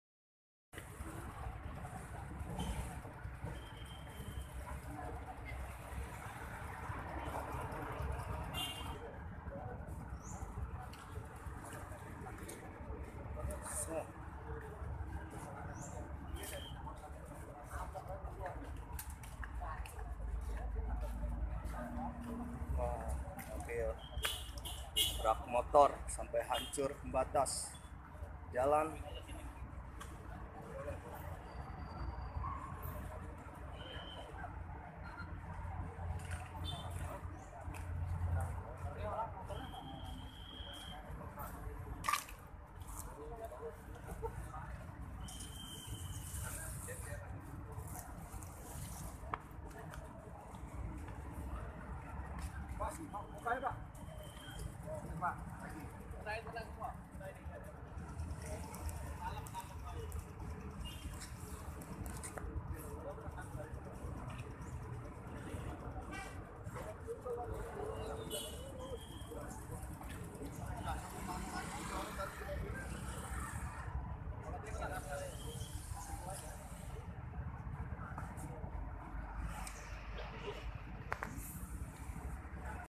PRIMAJASA TABRAK MOTOR DI PINTU MASUK TERMINAL
Bus prima jasa tabrak motor dan dua orang kritis dan langsung d evakuasi warga dan pihak dinas perhubungan dan kepolisian setempat